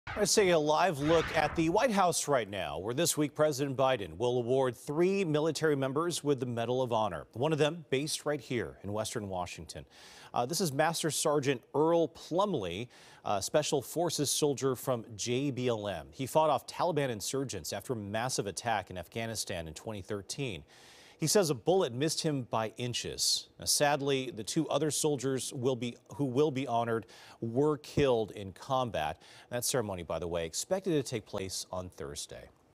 Three soldiers to receive Medal of Honor
A JBLM soldier and two other soldiers will receive a Medal of Honor.